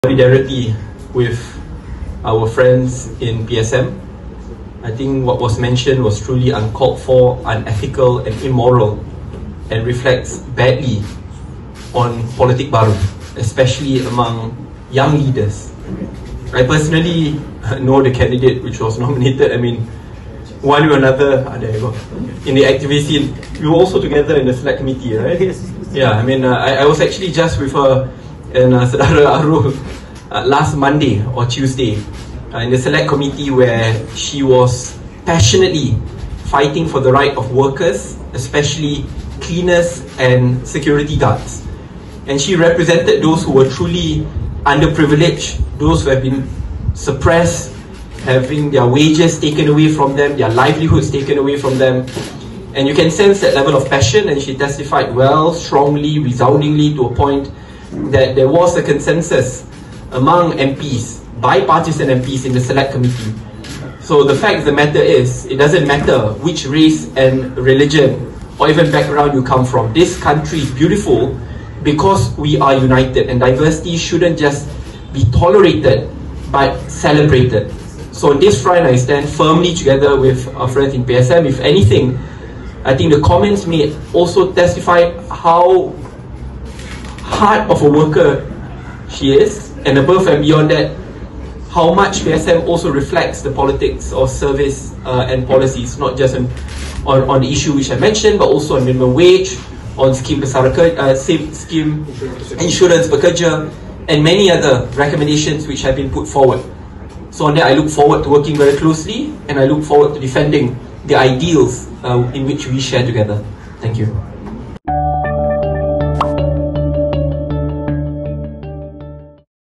抨蓝眼州议员种族主义言论不道德 赛沙迪：MUDA与社会主义党同在
大马民主联合阵线（MUDA）主席赛沙迪指出，人民公正党哥打胡姬州议员莫哈末纳兹旺对社会主义党（PSM）的言论，是无礼且不道德的。

他认为，这凸显了我国新式政治，特别是年轻领袖的糟糕之处。

「事实上，你来自什么种族、宗教、背景，都不重要，正因我们团结一致才能凸显这个国家的美妙之处。」

MUDA今日与社会主义党举行合作协议签署仪式，当媒体询及此事时，赛沙迪如是回应。（摄影：徐慧美）